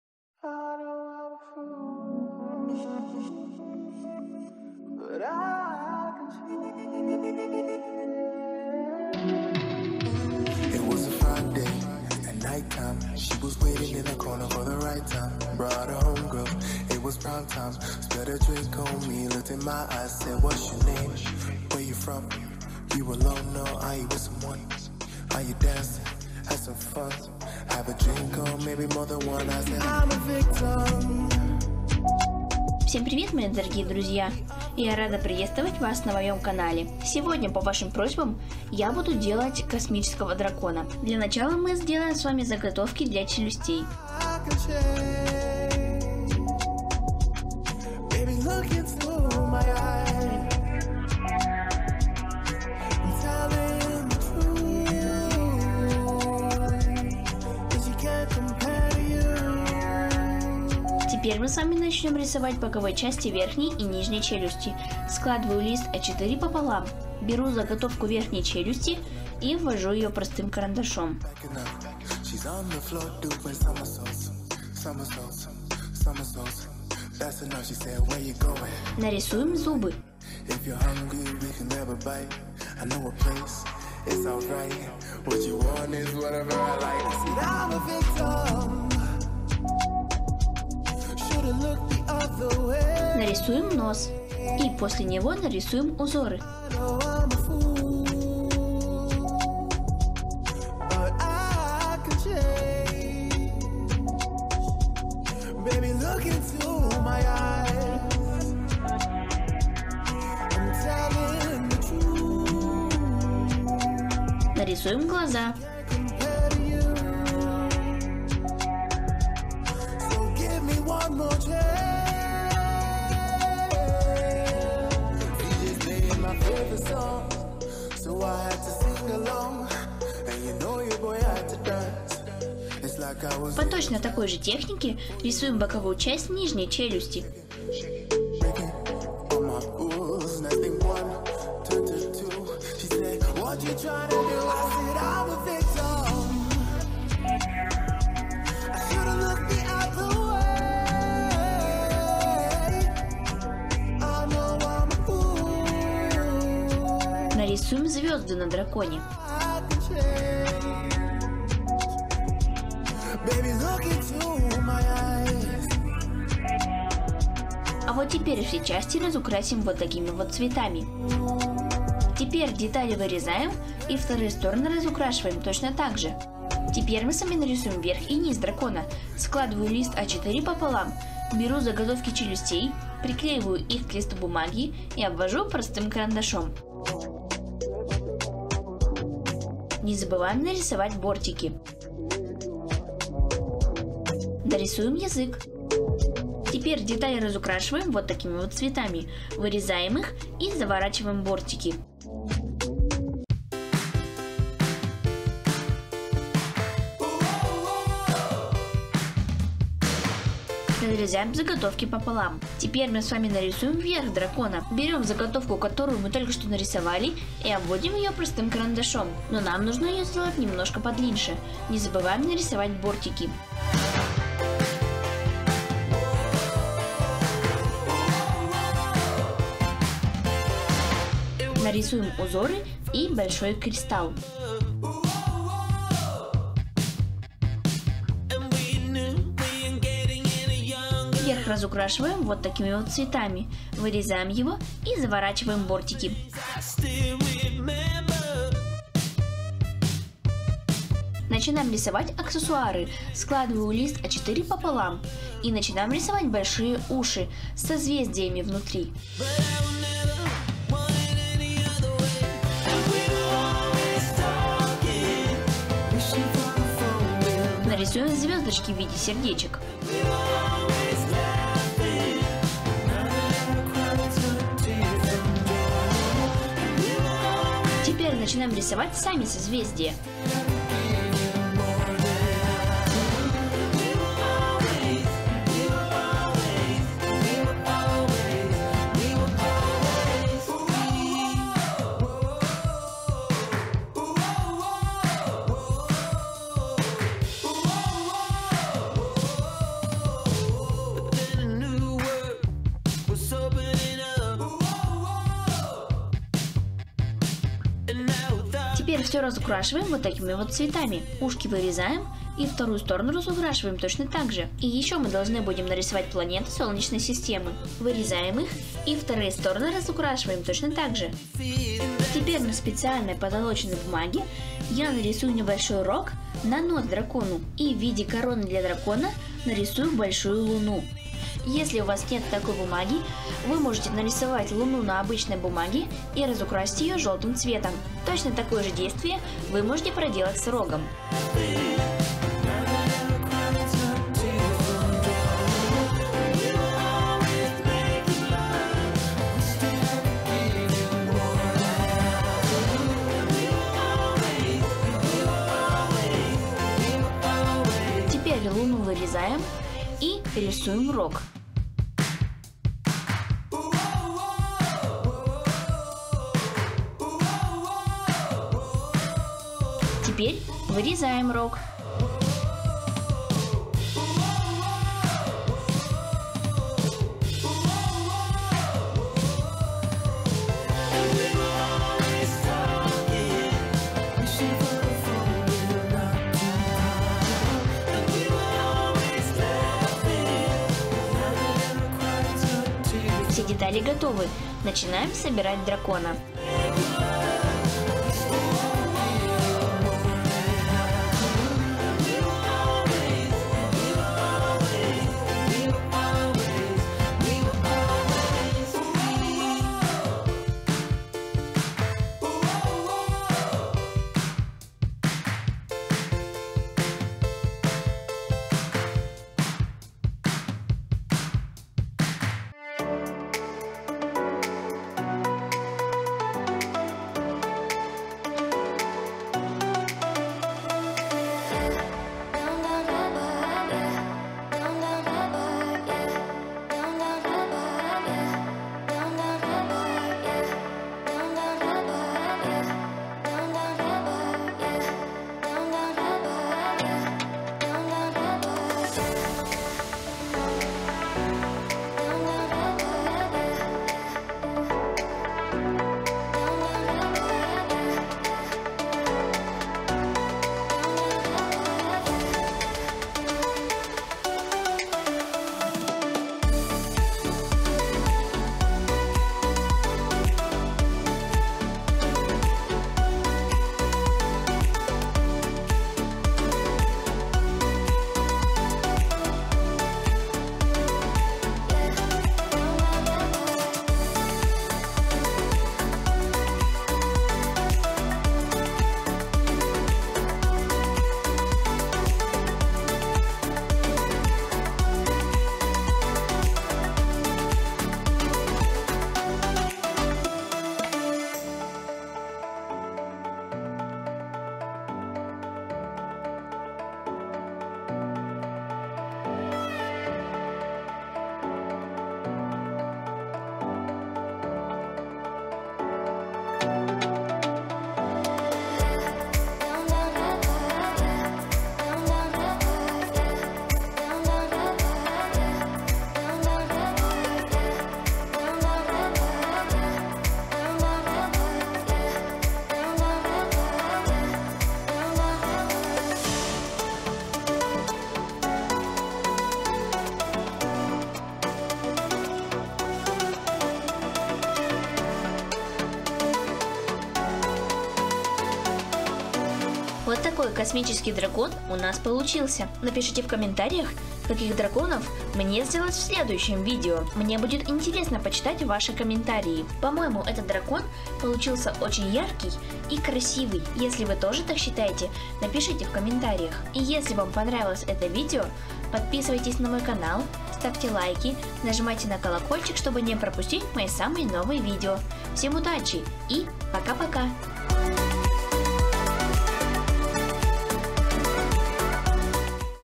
Как сделать Космического дракона из бумаги на руку. DIY Sofit Sun
Если вам нравится музыка в моем видео, пожалуйста, перейдите по ссылкам в описании ниже!

Всем привет. Сегодня я покажу как сделать Космического дракона из бумаги на руку. Смотри, как сделать бумажного дракона на руку!
Давайте вместе со мной сделаем такого дракона?!

 Для того чтобы сделать такого дракона, нам понадобится:
- Бумага размером А4
- ножницы
- маркеры
- клей

🔸 Большое спасибо всем, кто меня поддерживает!
🔸 Творческих вам успехов и вдохновения!